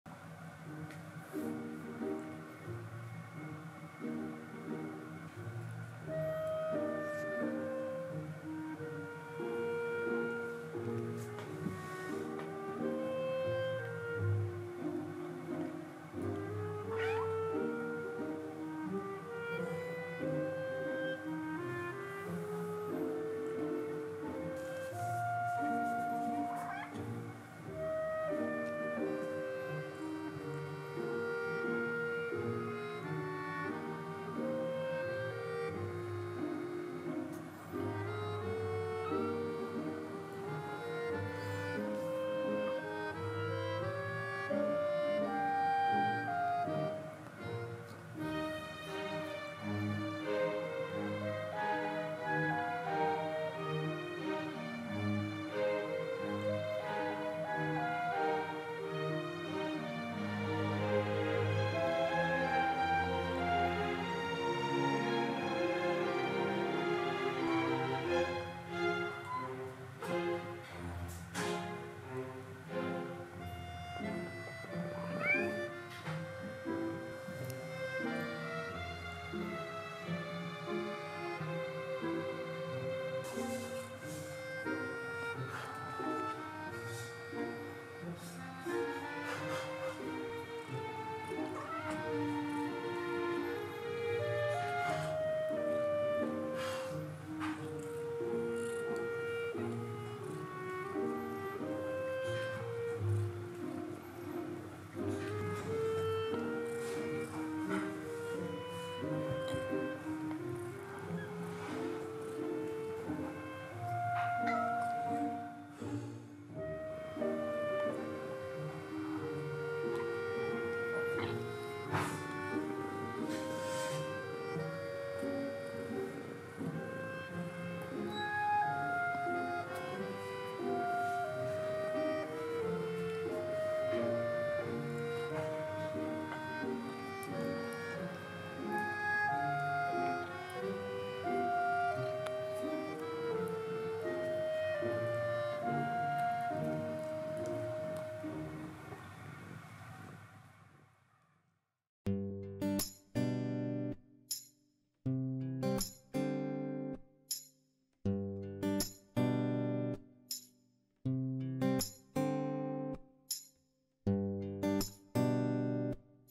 I put the kittens in a wheelbarrow and they were too cute
Have a look at the daily life of my cats.

□ Coco
Grandfather / Munchkin / Black and White

□ Mimi
Grandmother / British Shorthair / Golden

□ Kiki
Daughter / Munchkin / Brown tabby